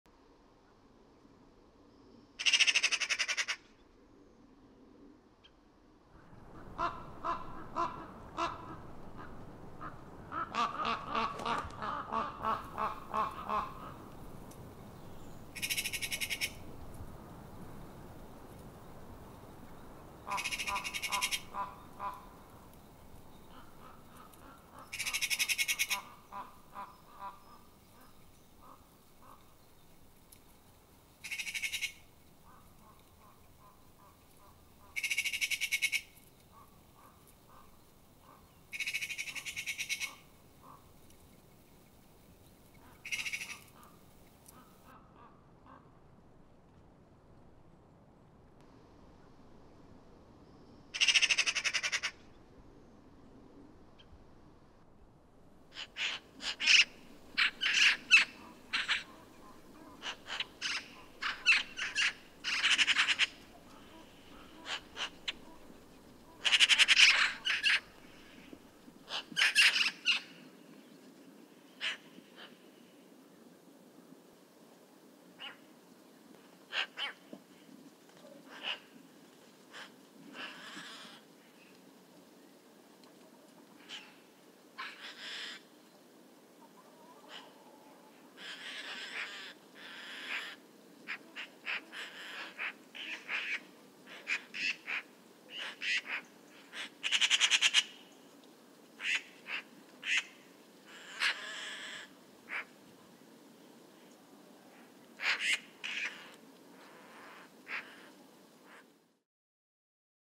The Eagle and the Magpie
Magpies trying to distract the Eagle by pecking its tail. Canon R3 and Canon 100-500mm 4K C LOG 3